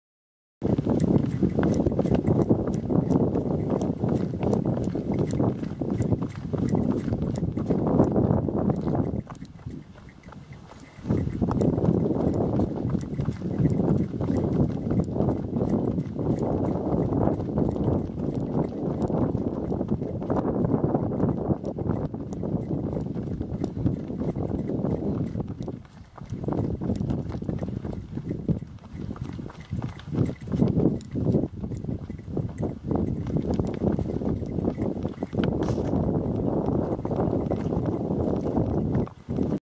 第1回蔵王温泉スカイトレイル 熊野岳
再び赤く荒涼とした火山のトレイル、熊野岳付近に戻ってきた。往路より増えたハイカーさんが声援を送ってくれる。とにかく頂上付近は気温が低く、走っていても寒いくらいだ。